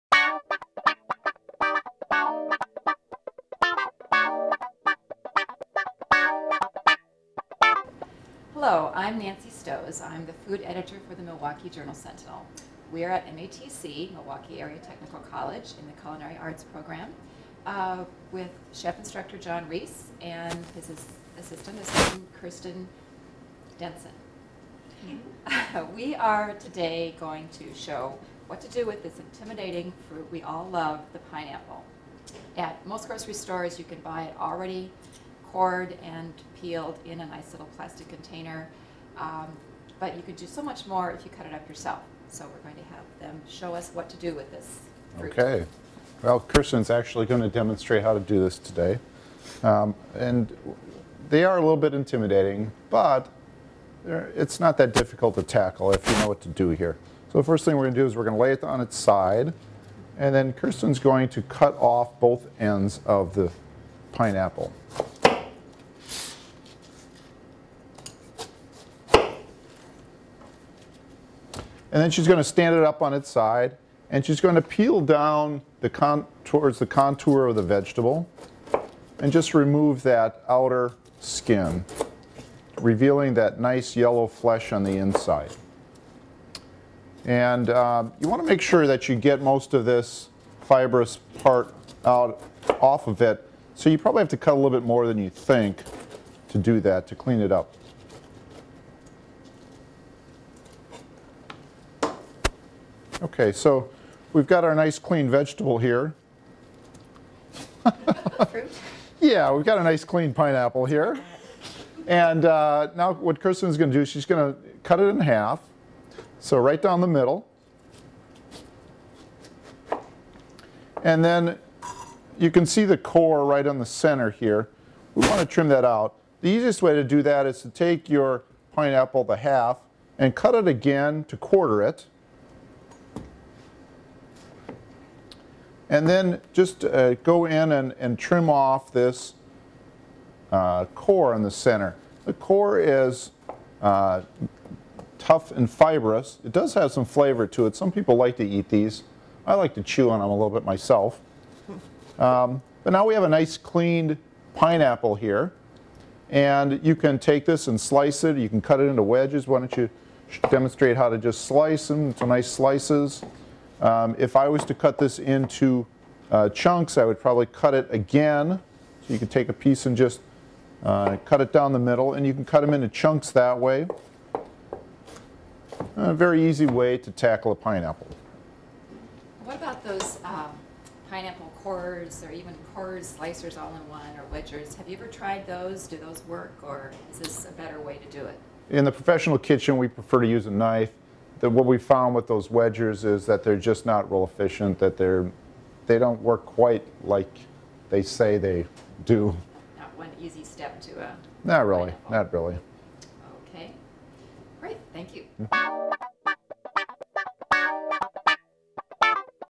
Counter Points: Slicing pineapple
This week on Counter Points, learn how to slice a pineapple. Video by Alison Sherwood (9/24/08)